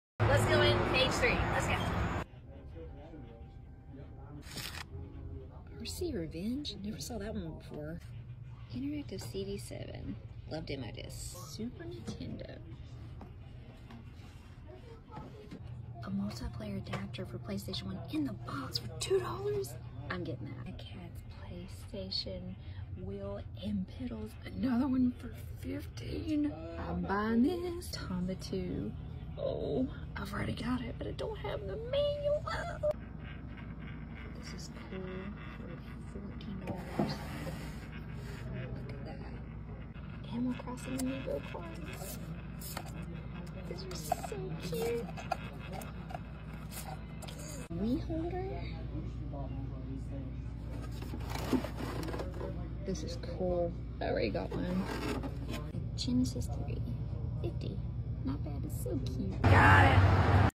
Game hunting at Page 3 Game Zone in Pikeville Kentucky!
I've been going to Page 3 in Pikeville Kentucky for probably over 10 years now.

Welcoming staff, great selection of video games and collectibles!

I always find something when I go in! If you are ever in the area, definitely check out the store!